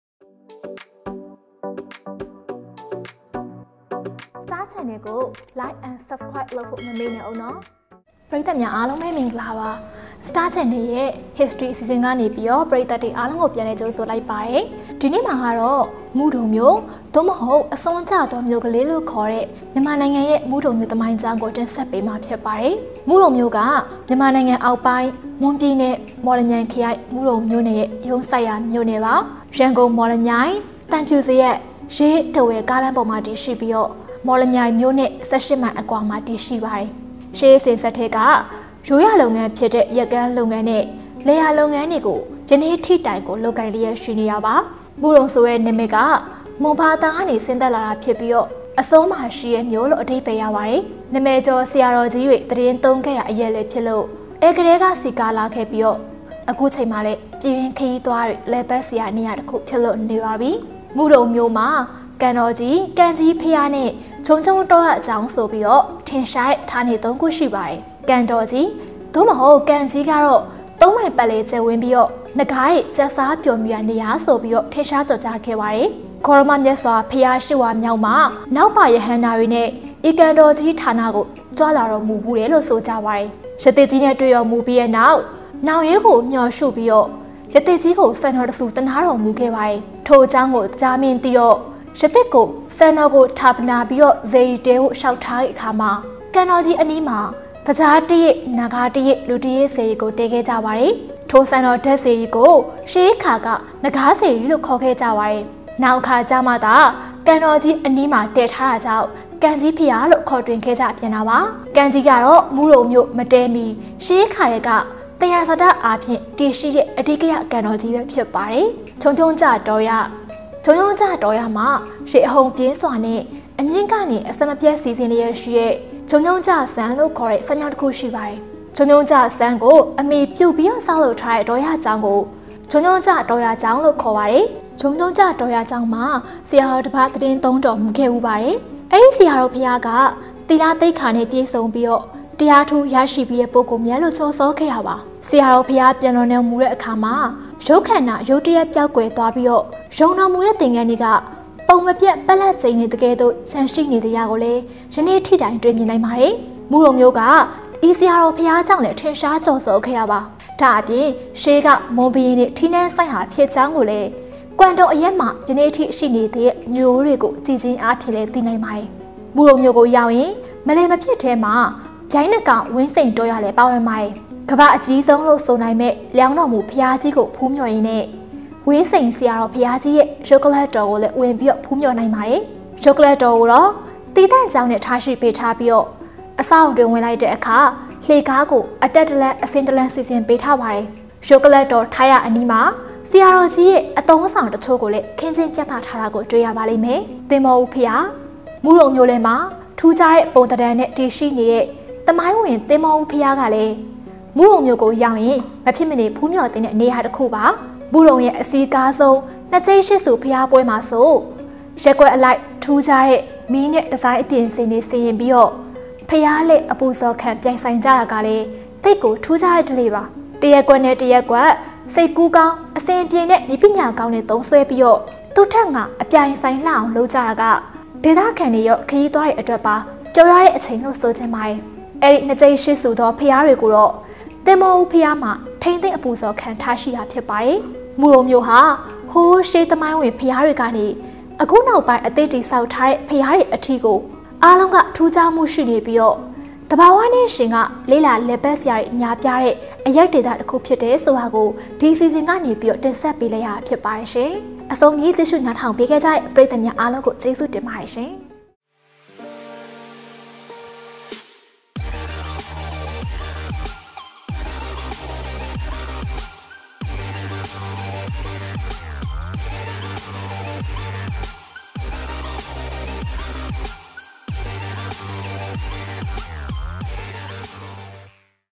မုဒုံမြို့ သို့မဟုတ် အစွန်းကျသောမြို့ကလေး
မုဒုံမြို့ဟာ မြန်မာနိုင်ငံ အောက်ပိုင်း မွန်ပြည်နယ် မော်လမြိုင်ခရိုင်၊ မုဒုံမြို့နယ်ရဲ့ ရုံးစိုက်ရာ မြို့နယ်ဖြစ်ပါတယ်။ ရန်ကုန်၊ မော်လမြိုင်၊ သံဖြူဇရပ်၊ ရေး၊ ထားဝယ် ကားလမ်းပေါ်မှာ တည်ရှိနေတာပါ။ မော်လမြိုင်မြို့နဲ့ ၁၈ မိုင်အကွာမှာ တည်ရှိတာပါ။ မုဒုံမြို့ကတော့ ရှေ့အစဉ်အဆက်က ရိုးရာလုပ်ငန်းဖြစ်တဲ့ ယက်ကန်းလုပ်ငန်းနဲ့ လယ်ယာလုပ်ငန်းတွေကို ယနေ့တိုင် လုပ်ကိုင်လျက်ရှိပါတယ်။

မုဒုံဆိုတဲ့ နာမည်ဟာ မွန်ဘာသာကနေ ဆင်းသက်လာတယ် ဆိုပါတယ်။ အစွန်းမှာရှိတဲ့မြို့လို့ ပြန်ဆိုကြပါတယ်။ နာမည်ကျော်ဆရာတော်ကြီးတွေ သီတင်းသုံးခဲ့ရာအရပ်လည်း ဖြစ်လို့ အဲ့ကတည်းက စည်ကားလာခဲ့ပြီး ခုချိန်မှာလည်း ပြည်တွင်းခရီးသွားတွေ လည်ပတ်စရာနေရာ ဖြစ်နေပါပြီ။